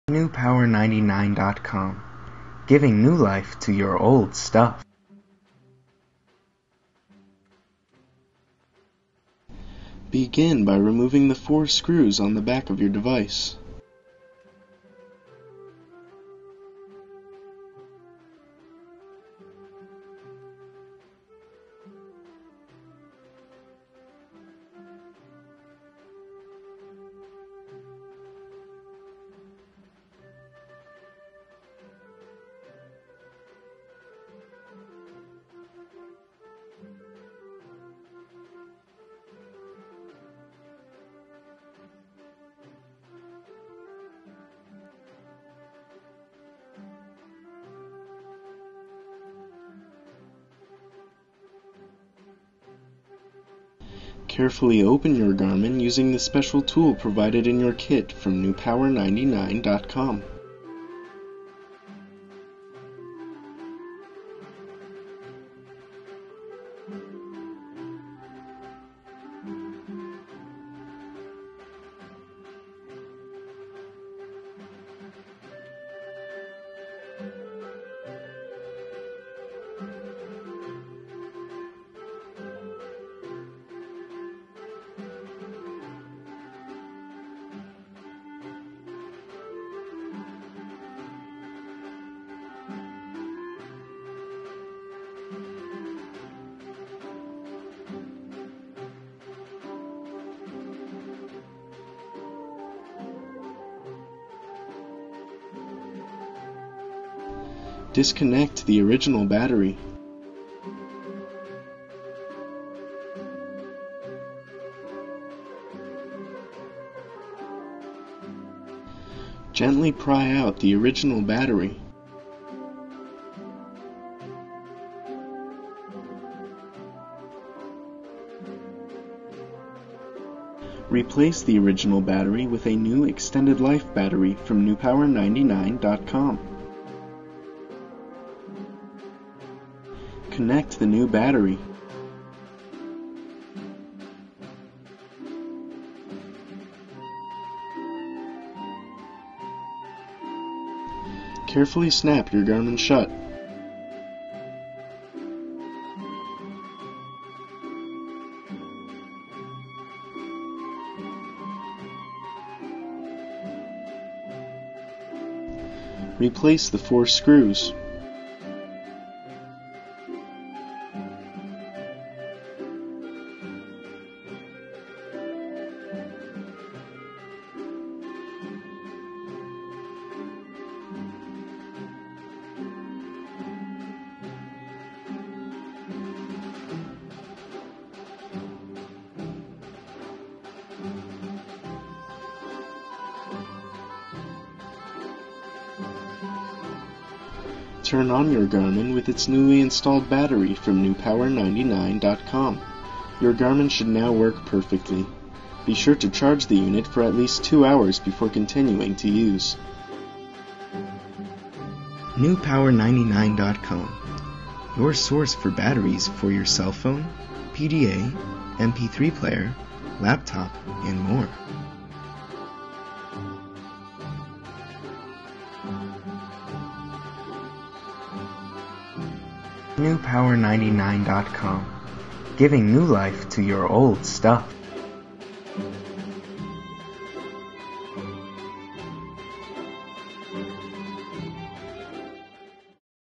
How to Replace Your Garmin Nuvi 2300LM Battery
Part of the Complete Installation Kit from NewPower99.com, available this video provides a step by step demonstration of the procedure for replacing the battery in your Garmin Nuvi 2300LM GPS.  The Installation kit contains a new high capacity battery, battery installation video, and necessary tools for battery replacement.  Includes one year money back guarantee!